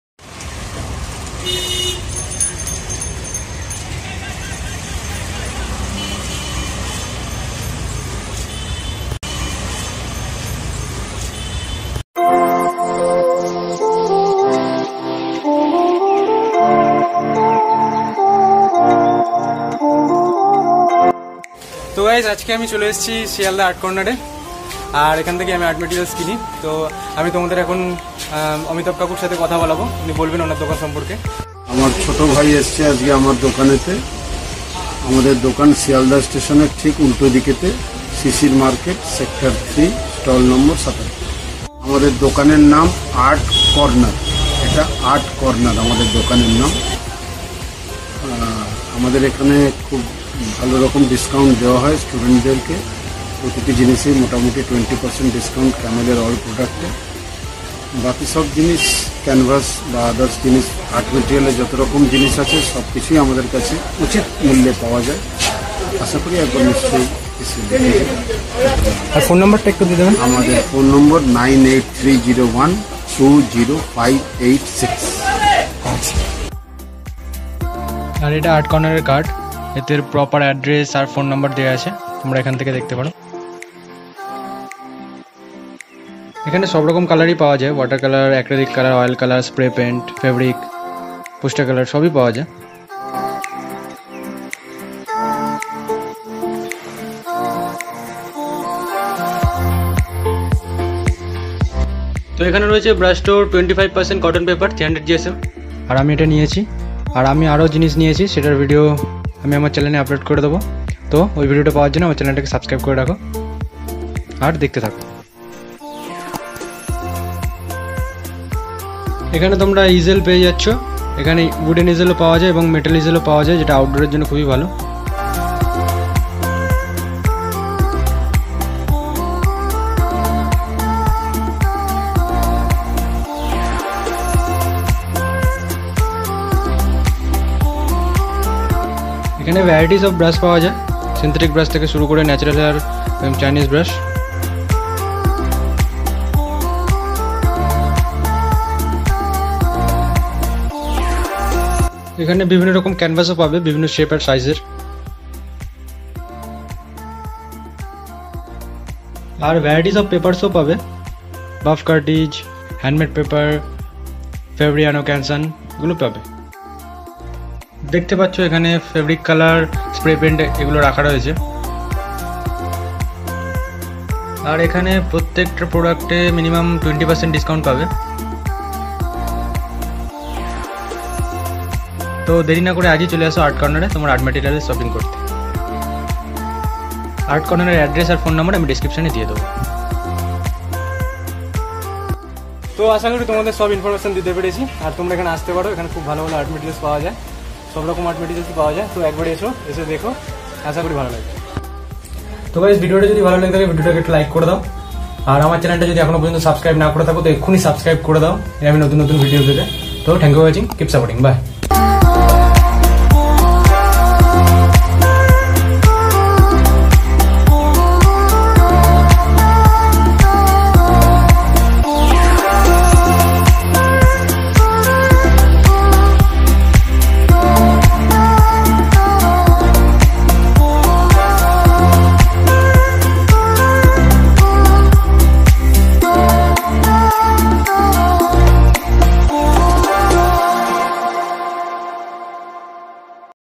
Art shop visit kolkata // Art Corner // Brought New Art Materials from Art Corner /
Art shop (Art Corner, Sealdah) visit kolkata // Bought new Art Materials from Art Corner Sealdah.

About this Video:
I visited Art Corner, Sealdah & Bought some art materials. Keep Supporting.

Art Corner Address :  SEC - 3, STALL NO 77, SISIR MARKET, SEALDAH.

Keep Supporting.